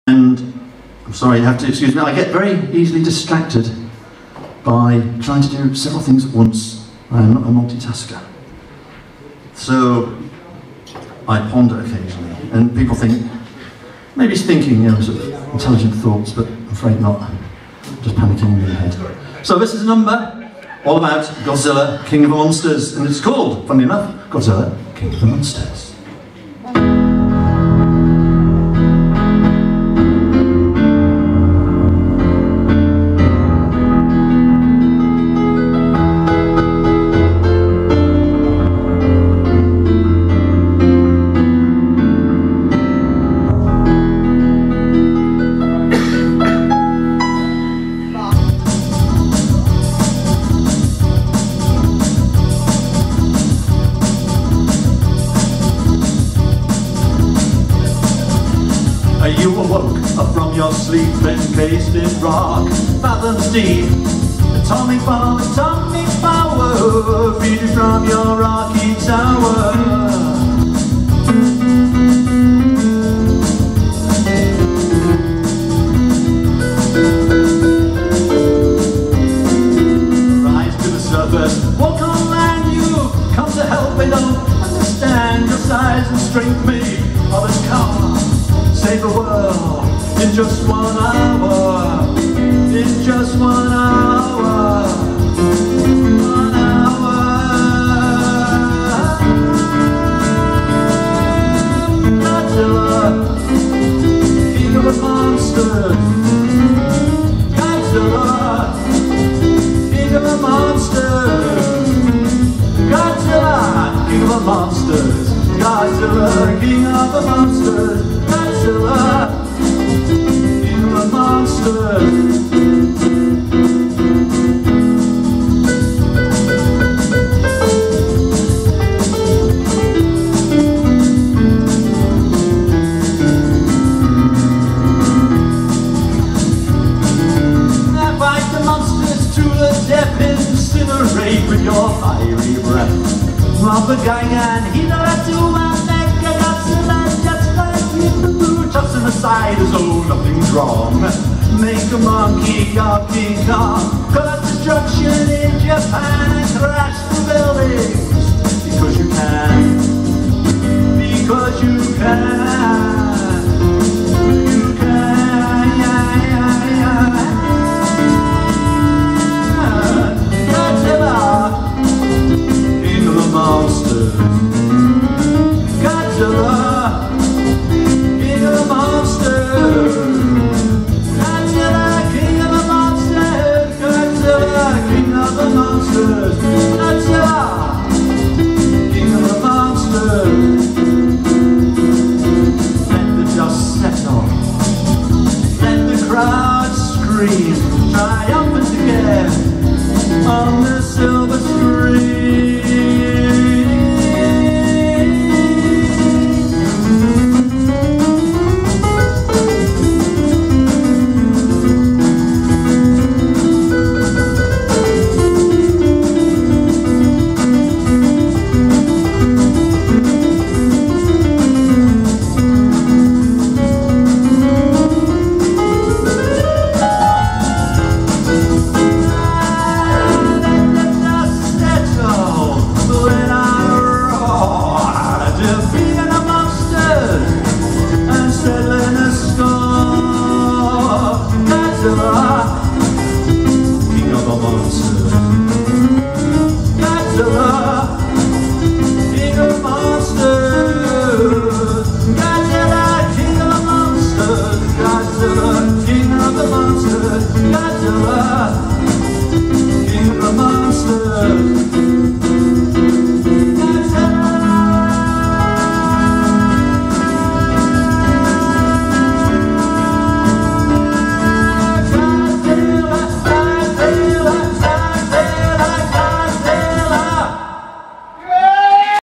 Graham Graham Beck
The second hilarious support act to get you in the mood for The Crazy Crazy World of Arthur Brown